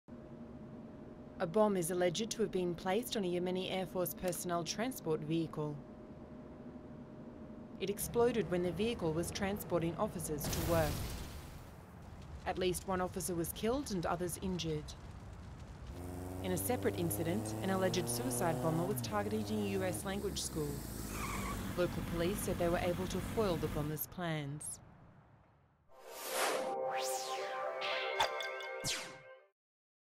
Yemen car bomb kills at least one air force officer in Sa'ana, scores injured
A bomb planted on a Yemeni air force personnel transport vehicle exploded, killing at least one officer and injuring scores others, Reuters news agency reported, based on information from state media.

Reuters reported that witnesses said: "as six people died when the device, planted on the vehicle, exploded and blew passengers' bodies into the street, but that higher toll could not be immediately confirmed."

The agency said that separately, Yemeni police said they foiled a plot to carry out a suicide bombing inside a U.S. language institute in Sanaa. An institute spokesman denied the report, saying a small incident involving the arrest of a motorcyclist trying to travel on a closed road that had been blown out of proportion.

The impoverished country, situated next to the world's top oil exporter Saudi Arabia, is battling a number of armed groups, including al Qaeda fighters who Washington said this month could be planning attacks.

Yemen's security personnel have been targeted by Islamist militants although most of the attacks have taken place in the largely lawless south and east of the Arabian Peninsula state.

Regarding Sunday's bombing, the air force spokesman told Saba news agency: "This cowardly terrorist act resulted in the martyrdom of one person and injured a number of others, several of whom are in a serious condition. They have all been transferred to hospital."

Saba named the dead man as Sergeant Mohammed al-Shaghdari.

Air force officer Ameen Saree, who was among the first to reach the scene, earlier said the blast killed at least six people and wounded 26.

There was no immediate claim of responsibility.

Later on Sunday, a Yemeni police spokesman and a counter-terrorism official said security forces had detained a suspected al Qaeda militant trying to infiltrate the Yemen-American Language Institute (YALI) on a motorcycle.

The police spokesman said the man was found to be carrying an explosives belt and was detained when he tried to enter the compound posing as a student seeking to register for English courses. Another official said the man had been referred to the counter-terrorism department for further questioning.

Source: Reuters